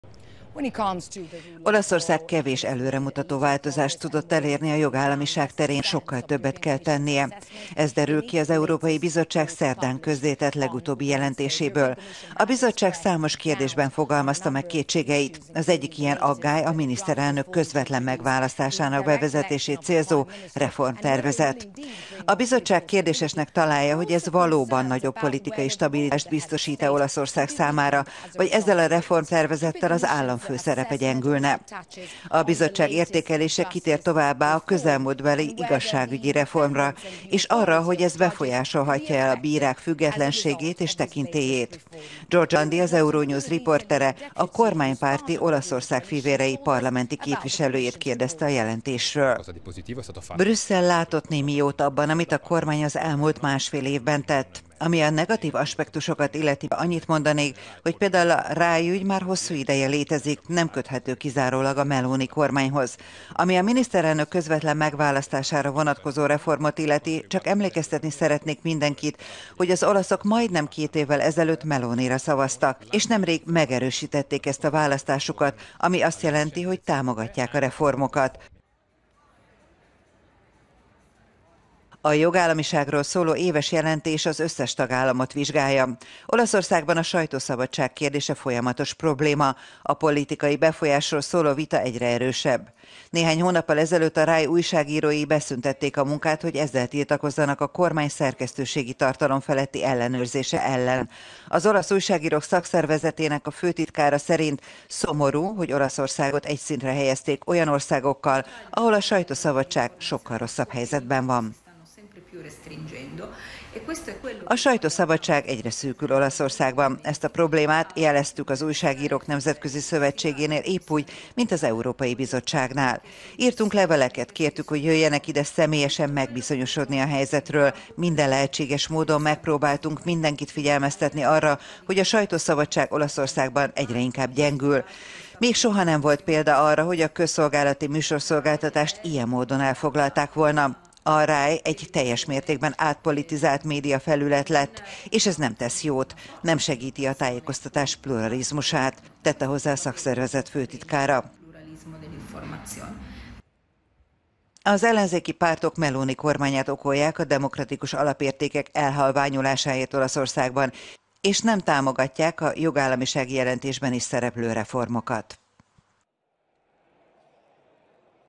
Baj van a sajtószabadsággal Olaszországban az Európai Bizottság szerint
Olaszország kevés előremutató változást tudott elérni a jogállamiság terén, sokkal többet kell tennie. Ez derül ki az Európai Bizottság szerdán közzétett legutóbbi jelentéséből.